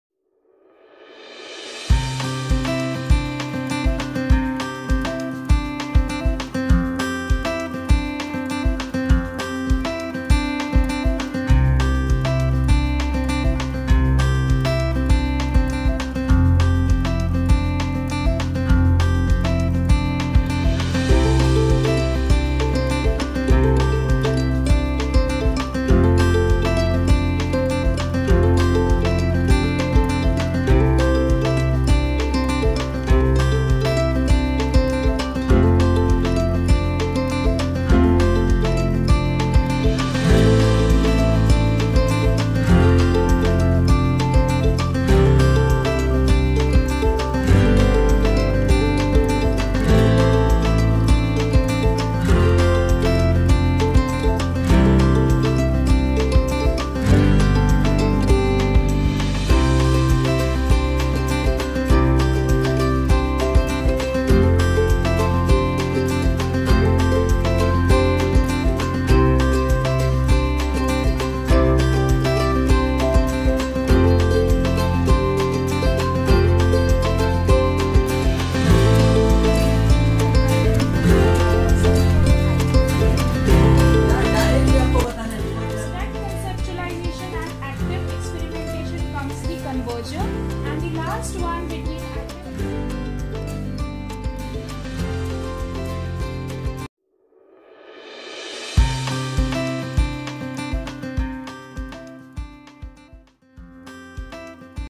Full STEAM ahead immersive training programme for teachers of Anjuman I Islam
Video from Anjuman I Islam's Begum Sharifa Kalsekar Girls English School